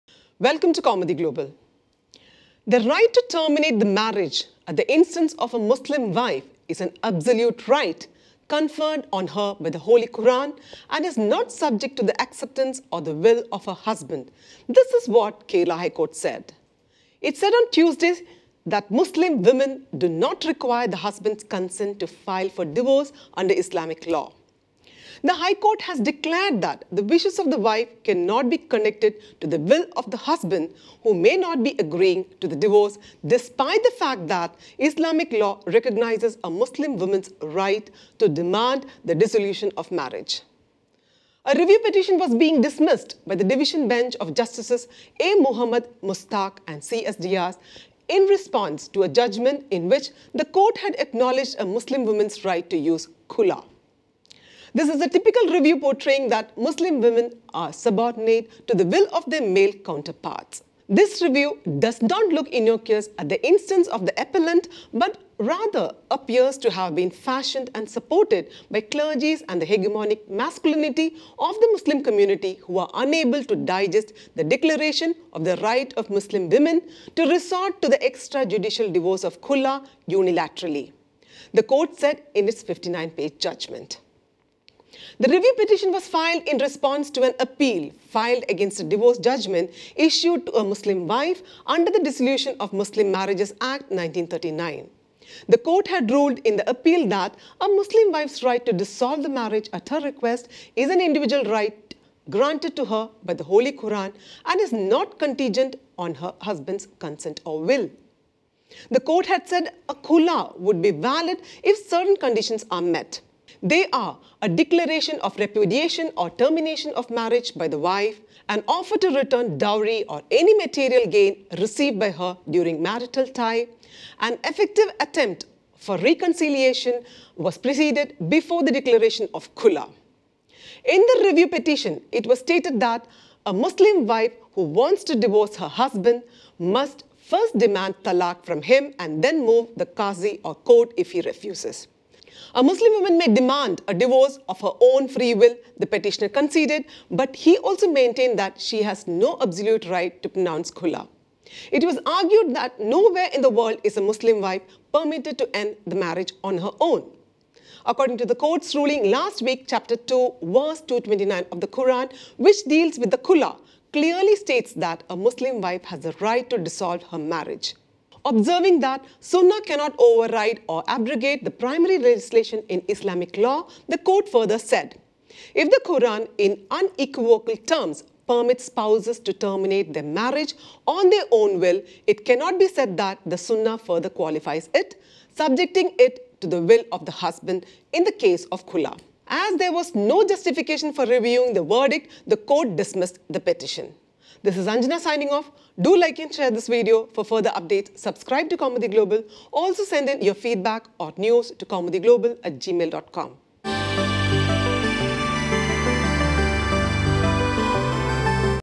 Wishes of the Wife cannot be connected to the Will of the Husband: Kerala HC
The right to terminate the marriage at the instance of a Muslim wife is an absolute right, conferred on her by the Holy Quran and is not subject to the acceptance or the will of her husband. This is what Kerala High Court said. It said on Tuesday said that Muslim women do not require the husband's consent to file for divorce under Islamic law. The High Court has declared that the wishes of the wife cannot be "connected to the will of the husband," who may not be agreeing to the divorce, despite the fact that Islamic law recognises a Muslim woman's right to demand the dissolution of marriage.